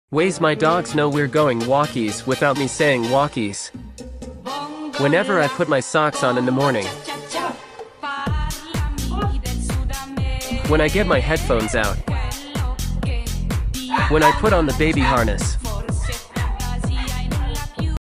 Ways My Mini Huskies Know We Are Going WALKIES Without Me Telling Them
For today's post, we read Skye and Copper about a heroic dog called Eva who saved her owner's life after a scary mountain lion attack. What a brave doggo! Copper asked for a scarier story and he sure got one with this tale.